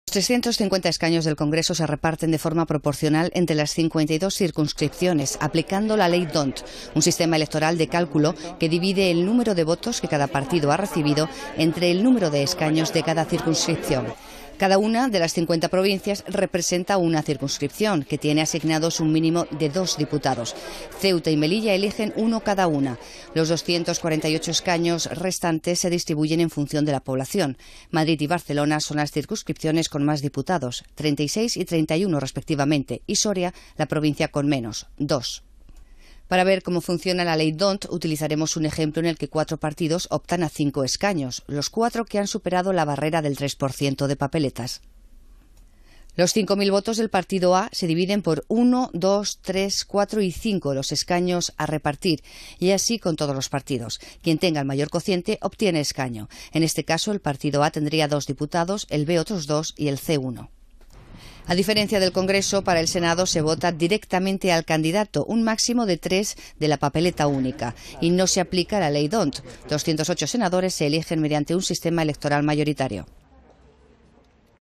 Cómo funciona la ley D'Hondt, con la que se reparten los escaños en España
Los 350 escaños del Congreso se reparten de forma proporcional entre las 52 circunscripciones, aplicando la ley D'Hondt, un sistema electoral de cálculo que divide el número de votos que cada partido ha recibido entre el número de escaños de cada circunscripción y reparte los asientos en función de los cocientes obtenidos.